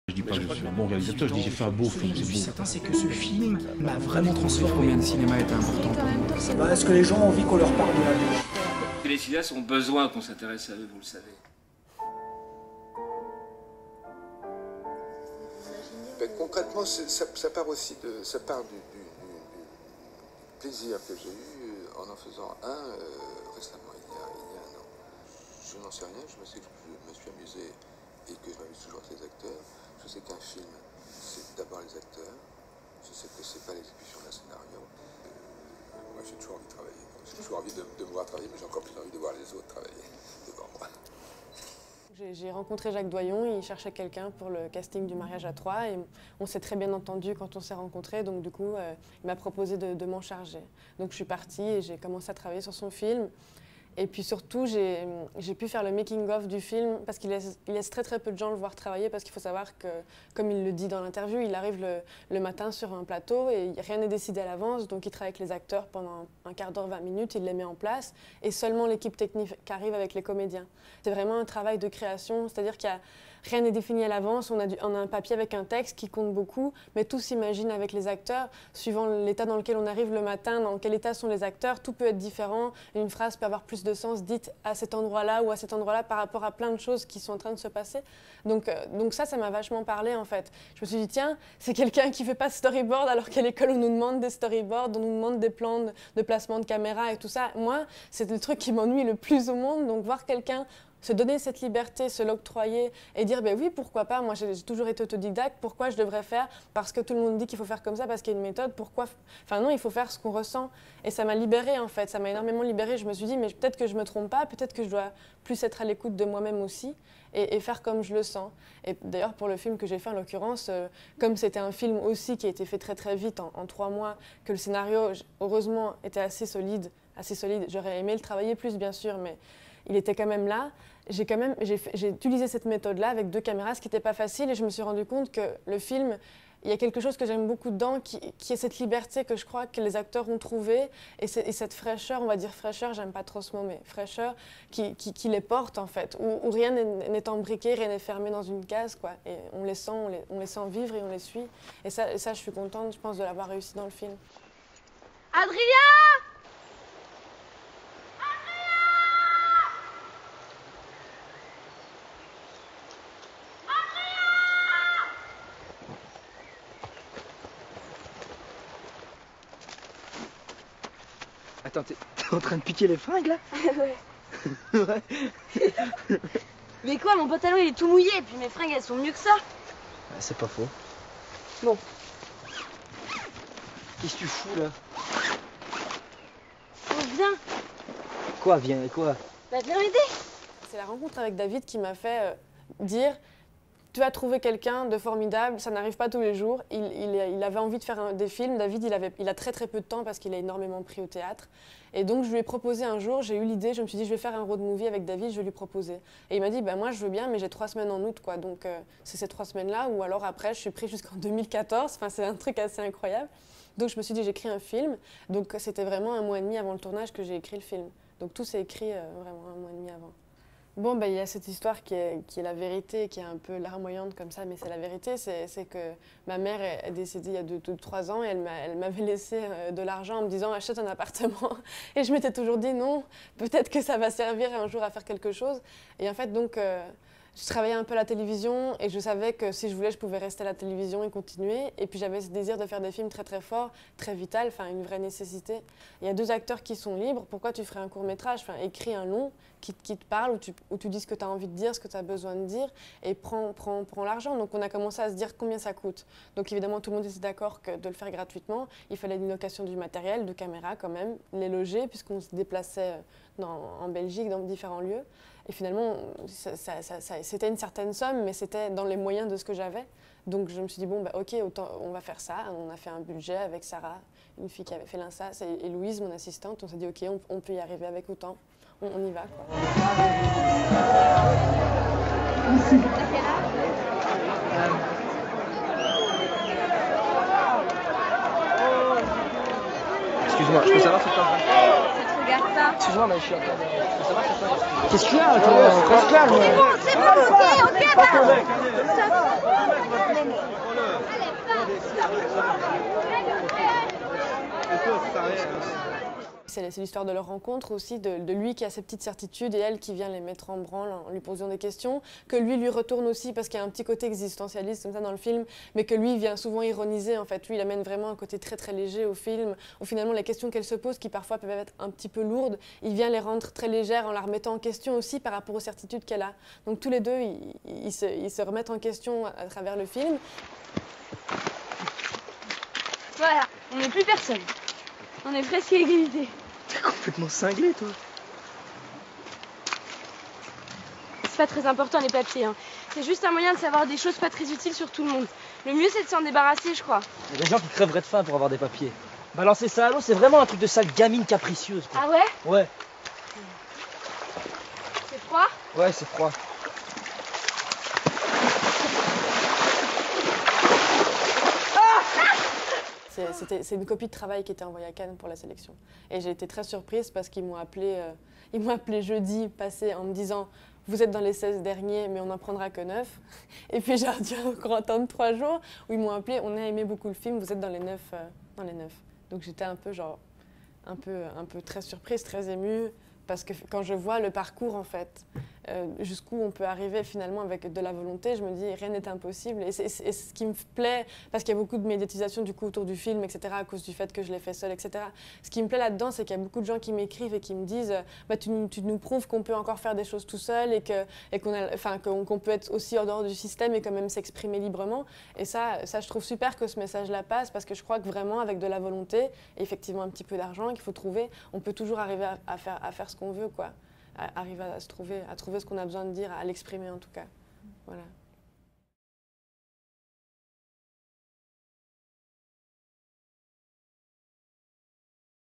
Amélie van Elmbt, la Tête la première, sélectionné à Cannes, ACID
Rencontre avec la jeune réalisatrice Amélie van Elmbt de la Tête la première, film entièrement autofinancé, sélectionné à Cannes, section ACID.